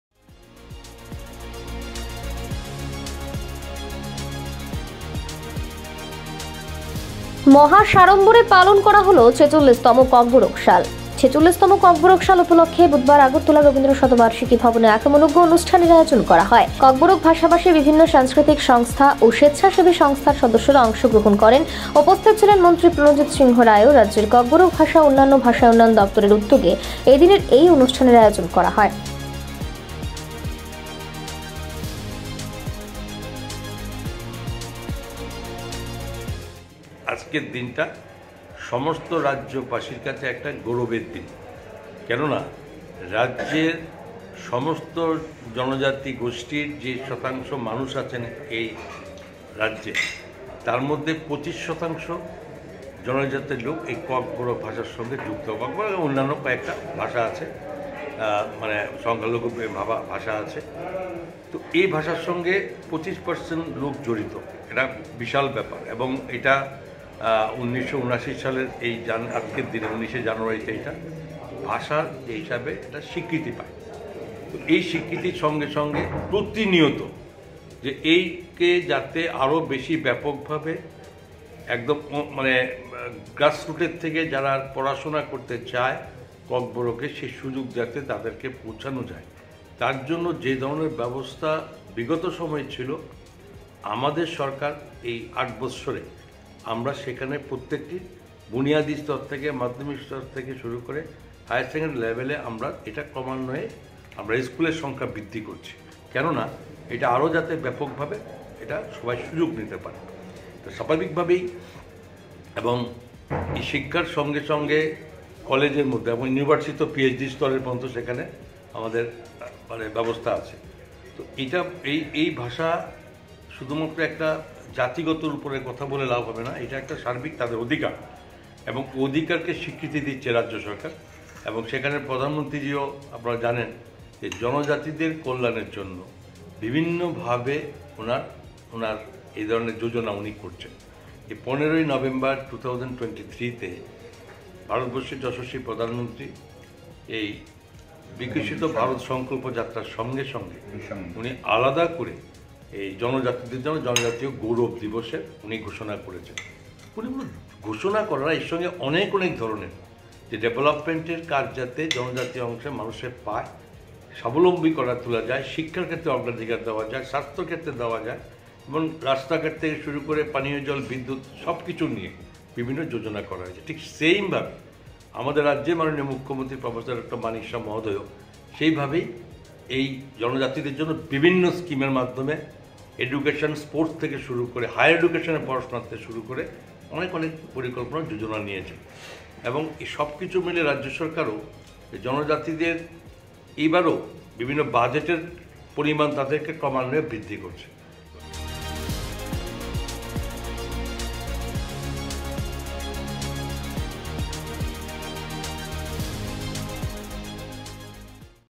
Agartala : ৪৬ তম ককবরক সাল উপলক্ষে রাজধানীতে বিশেষ অনুষ্ঠানের আয়োজন || VOICE 18
VOICE 18
Name of Production House: FLAXEN MEDIA WORKS